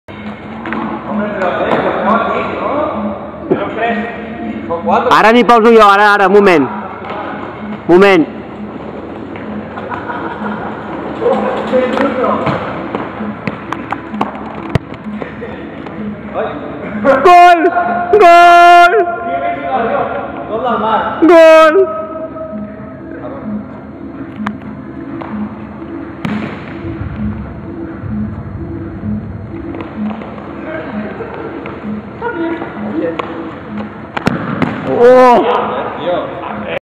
Delafé Ac2 Vs Las Trompetas de la Muerte "los ases del balón"
Partidito de los cracks antes de las pruebas de sonido Calonge